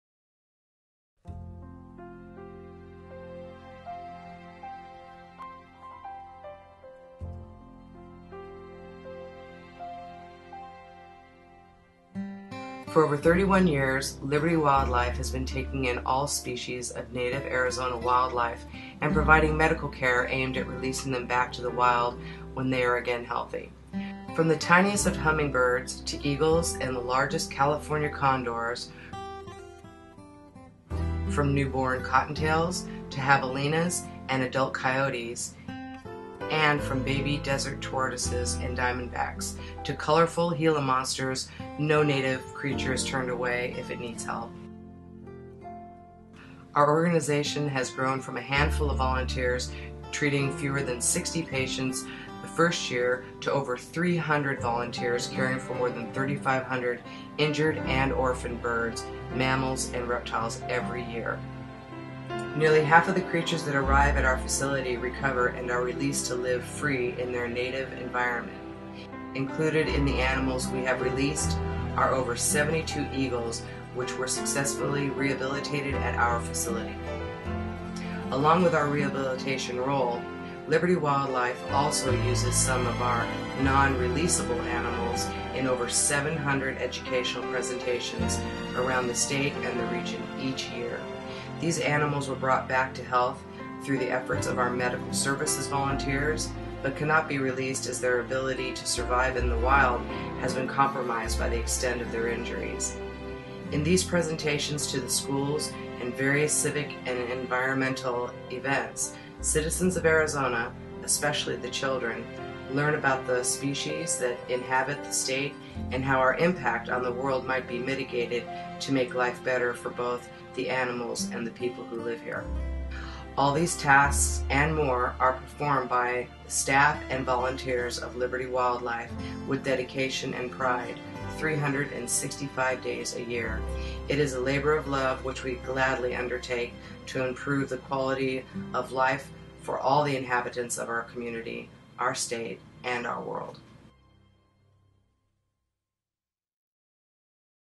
Liberty Wildlife Video
A brief history and introduction to Liberty Wildlife in Scottsdale, AZ